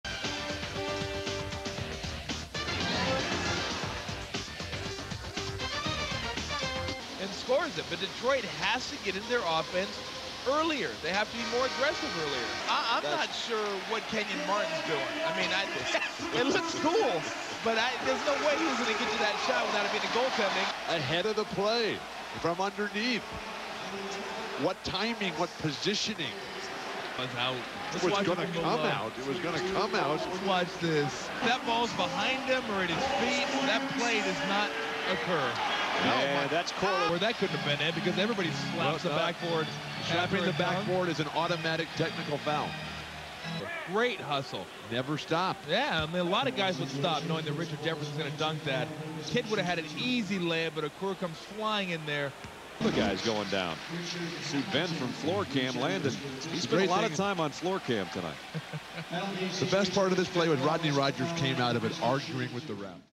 One Minute of Floor Cam Footage from a 2003 Pistons-Nets Playoff Series
In its first year of taking over the NBA broadcast rights from NBC, ESPN experimented with a handful of new features and gadgets, including Floor Cam, a lens embedded in the home team's hardwood floor. An innovation perhaps better in theory than execution, Floor Cam mostly served as a vessel for indecipherable action replays and awkwardly voyeuristic up-short shots of NBA players hopping about beneath the basket.

The footage above comes from the first three games of the 2003 Eastern Conference Finals between the Detroit Pistons and New Jersey Nets. The Nets swept the Pistons 4-0 en route to making their second consecutive appearance in the NBA Finals.